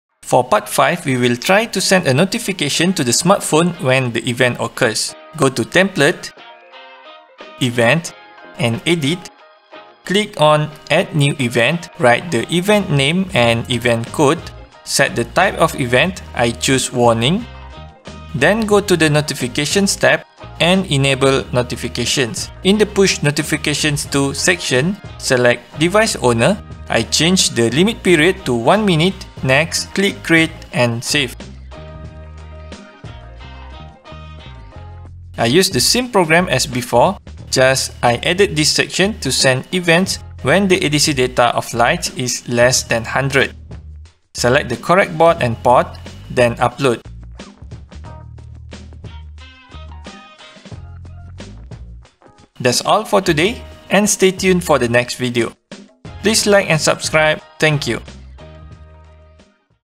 [blynk.io] Part 5 - Sending Notification
For part 5 we will try to send a notification to the smartphone when the event occurs. 😄

Idris
Cytron Technologies